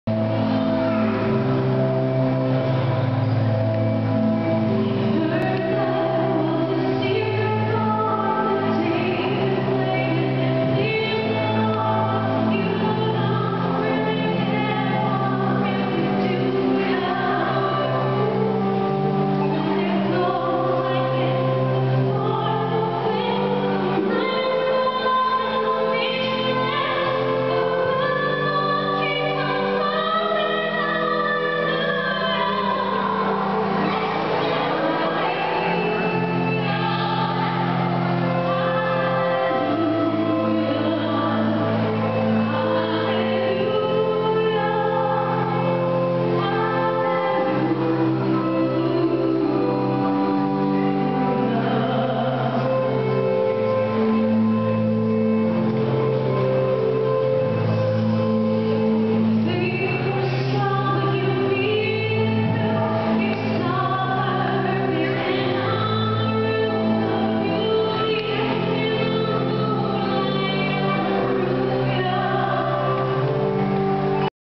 Hydraulophone concert (short record)
Played at the Royal Ontario Museum on August 7, 2011, Toronto, ON.
Artists: Ryan Janzen, Steve Mann, Larissa Koniuk.